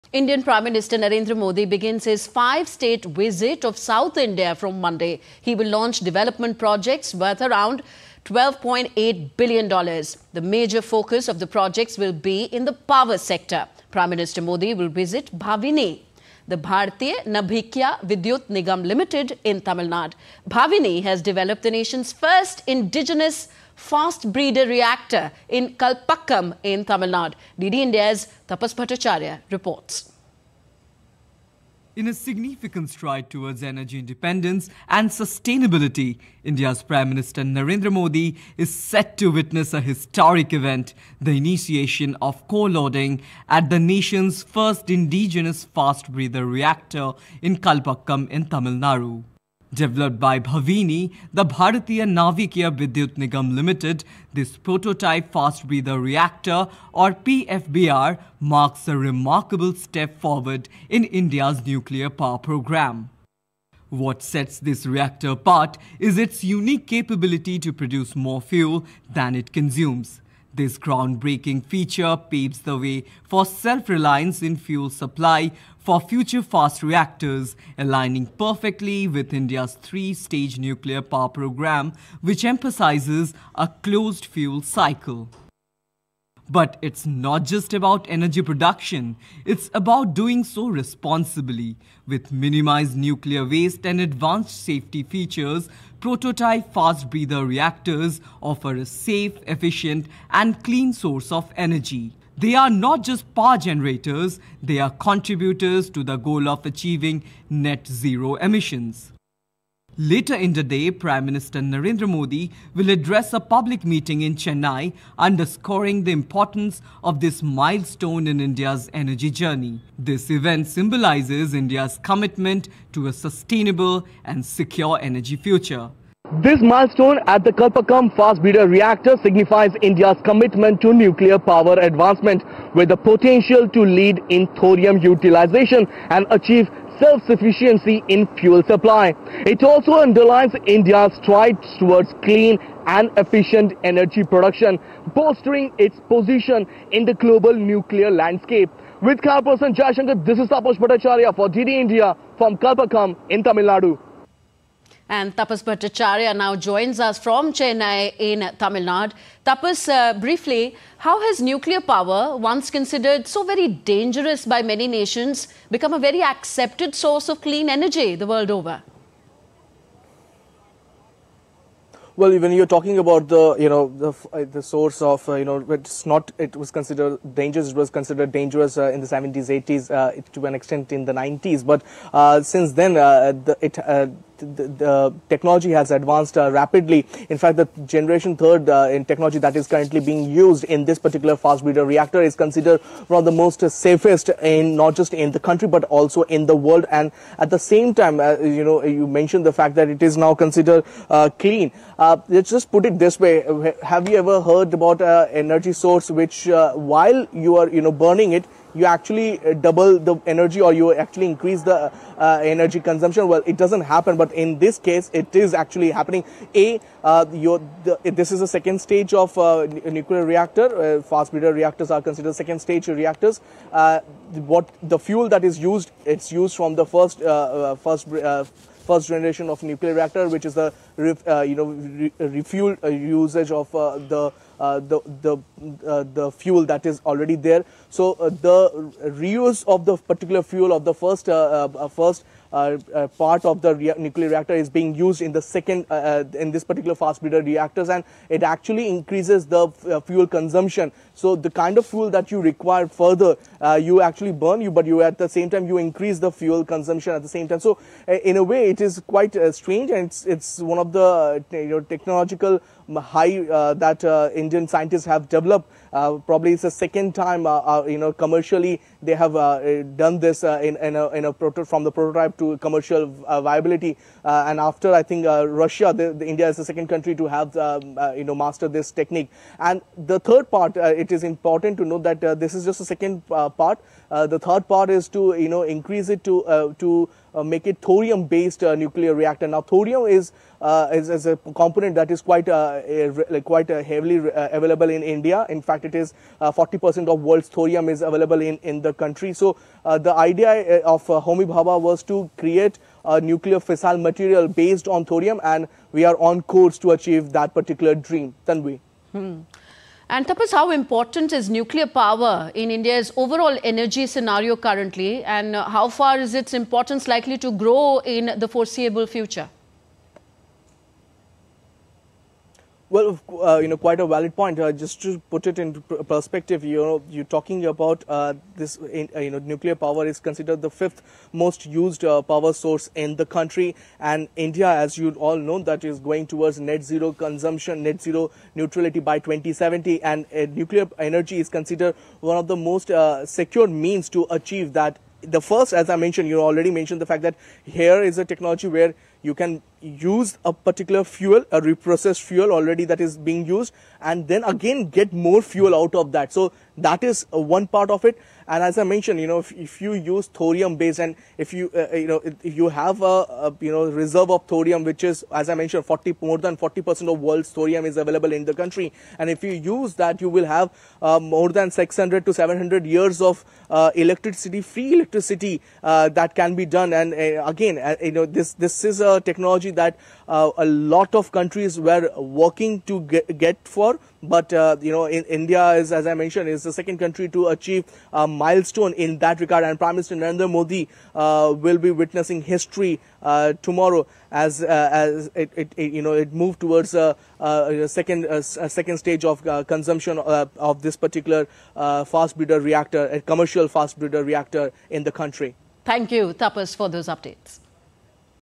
India's PM Modi to inaugurate indigenous nuclear reactor at Kalpakkam, Tamil Nadu
As a historic milestone in India’s nuclear power programme, Indian Prime Minister Narendra Modi to witness initiation of core loading of India’s indigenous Prototype Fast Breeder Reactor at Kalpakkam in Tamil Nadu on Monday.